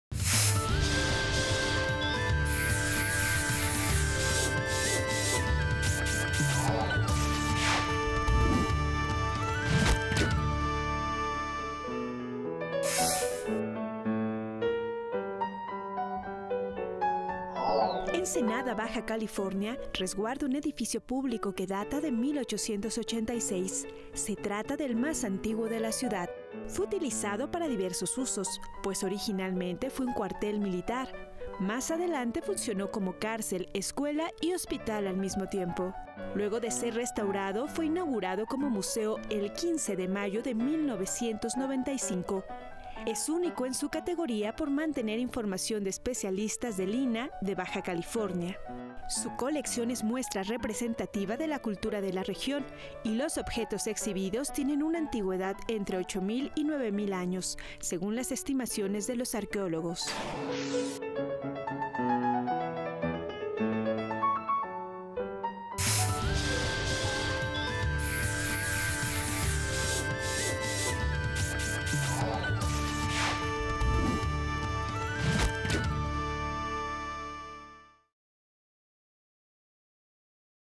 Museo Histórico Regional de Ensenada
Ensenada, Baja Califronia, resguarda un edificio público que data de 1886; se trata del más antiguo de la ciudad. Luego de ser restaurado, fue inaugurado como Museo el 15 de mayo de 1995, es único en su categoría por mantener  información de especialistas del INAH de Baja California